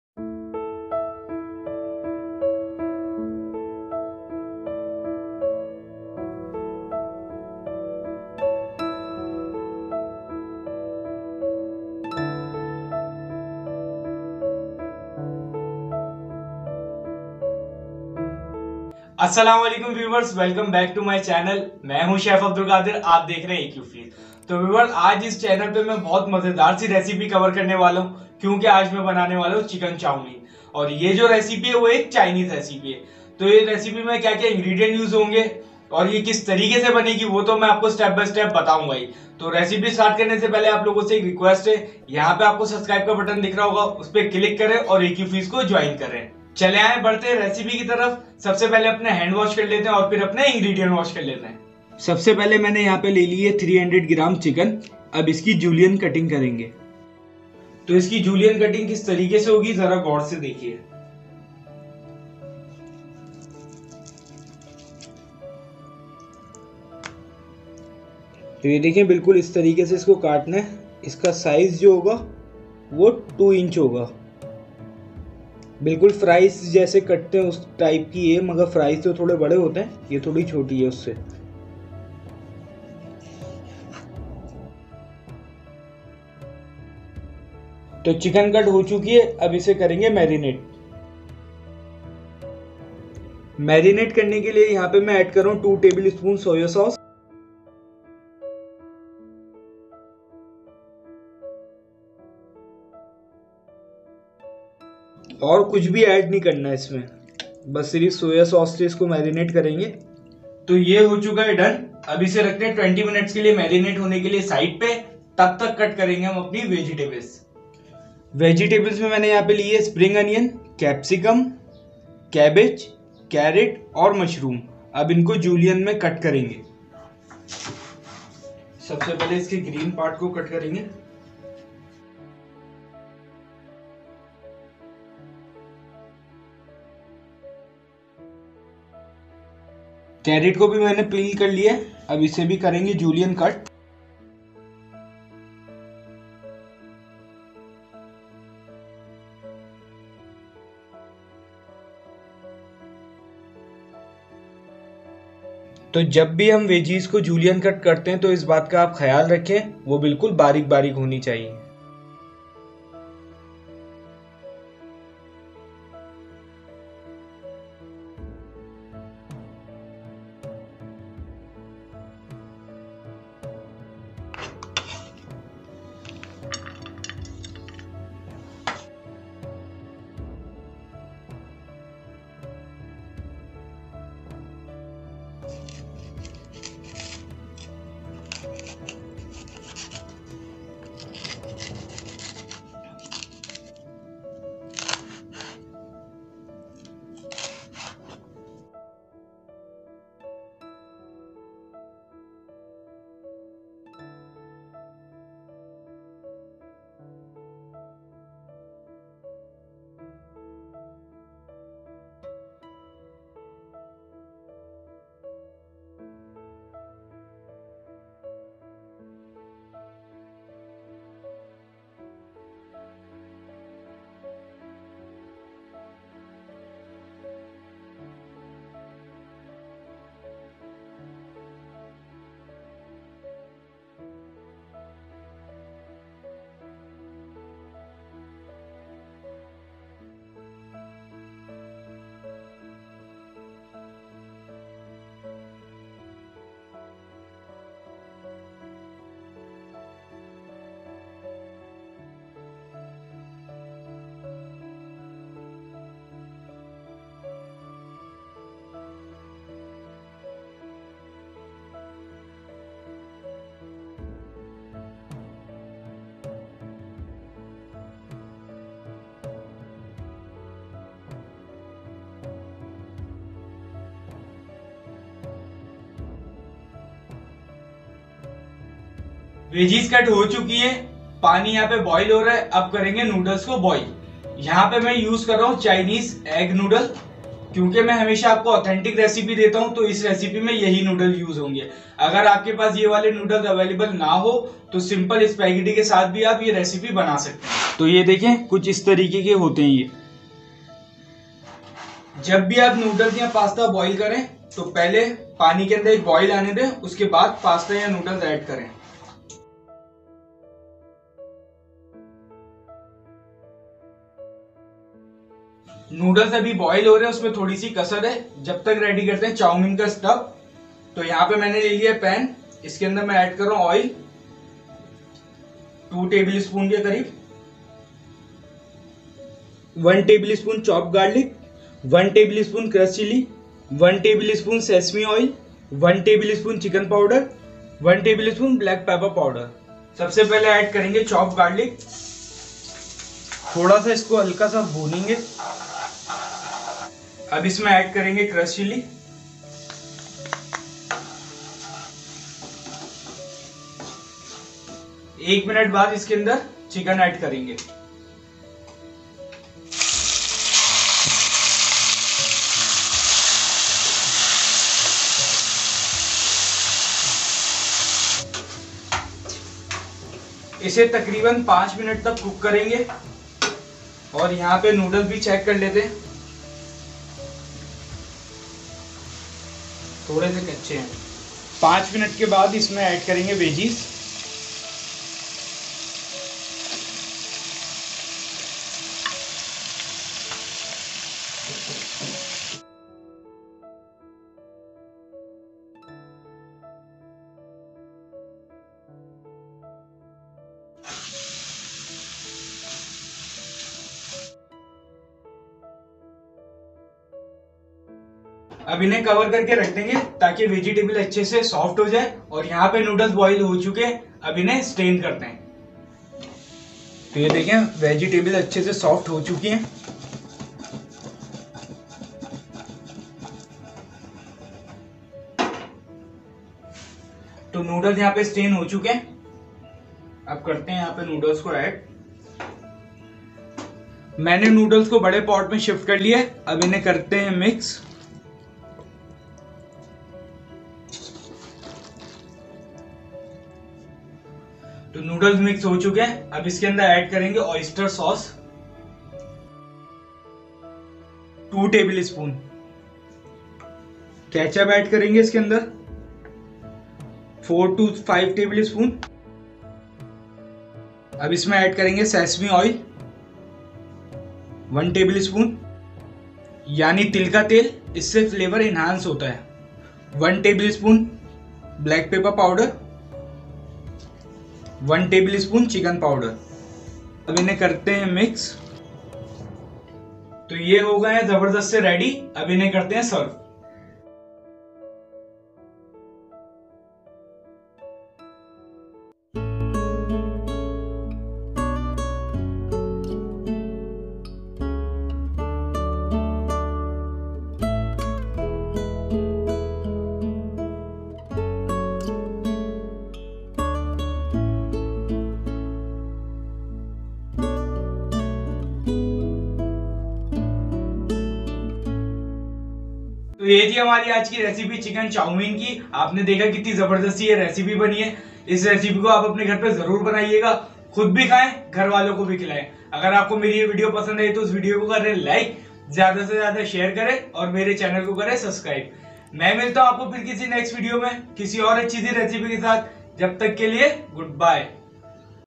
Chicken Chowmein Recipe | Chinese Noodles Recipe by Chef AQ
Hey Friends! Here's an energetic comeback with an Intriguing Chinese recipe. "Chicken Chow Mein"